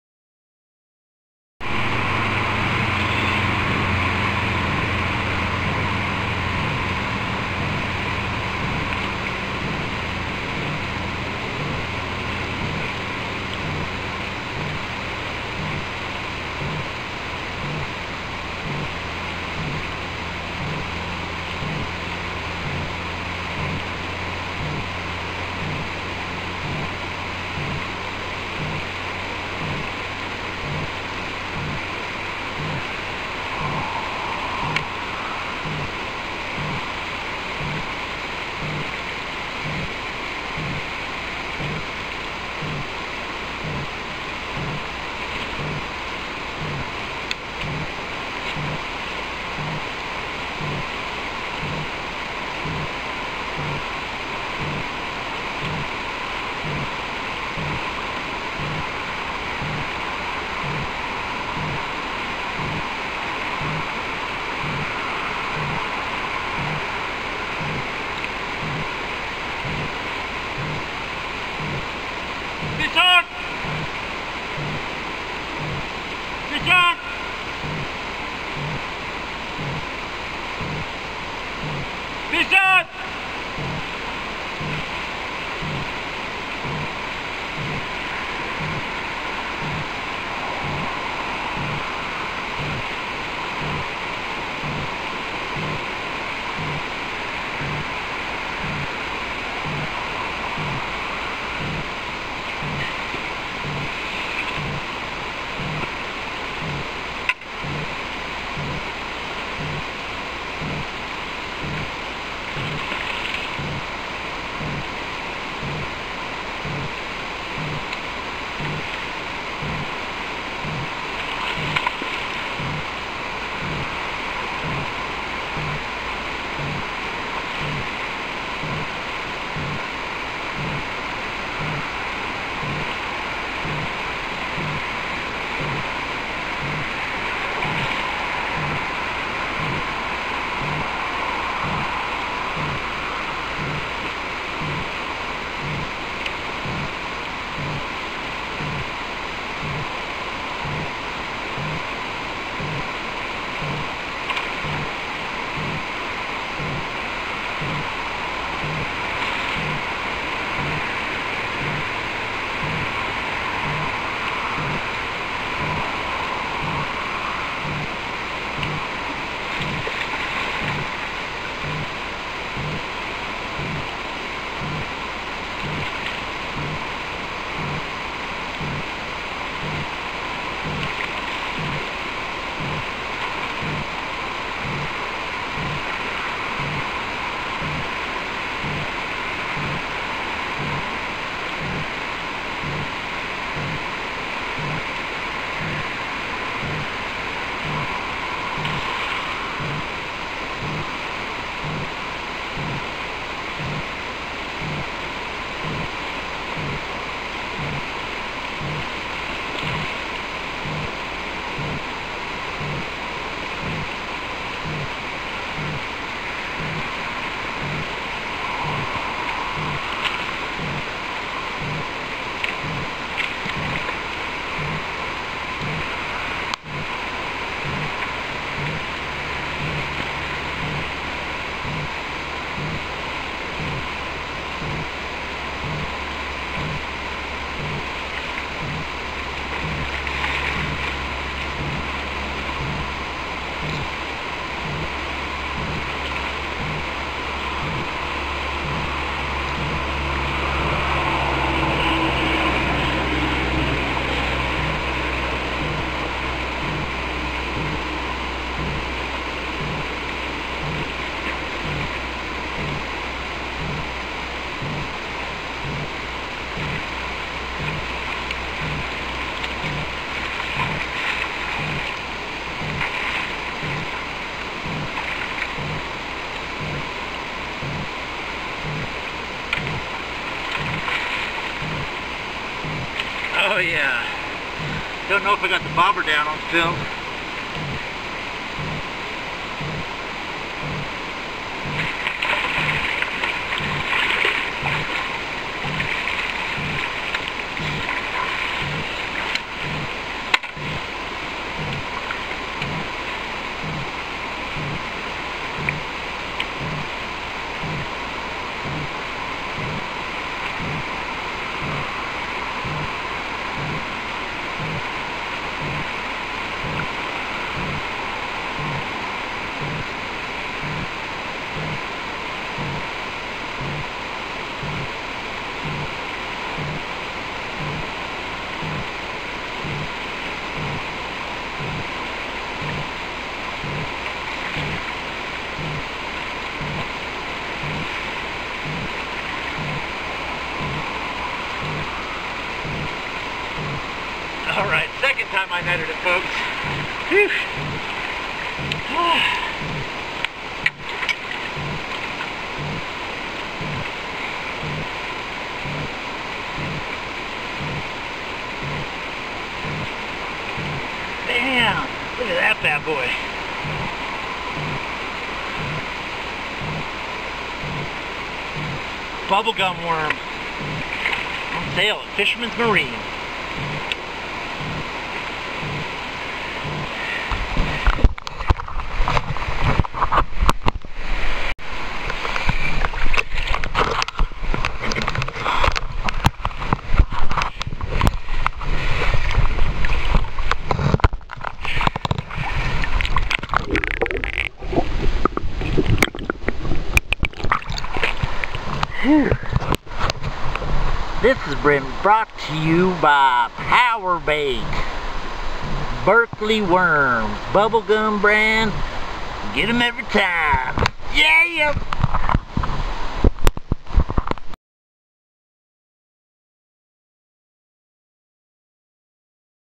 Davids Late March Hen 2013
This is a funny video of me catching my last winter steelhead on this particular river. @ 1:10  if you have a good eye you can see the bobber go down in the top left hand of the screen. I then yell fish on 3 times to my friend who is way down river. The fish almost got away when I was trying to get a quick pick. She put up a good fight and as you can see the fish took off immediately un-phased by our little dance on the river! This was my last attempt for winter steelhead on this particular river system.Good winter season this year.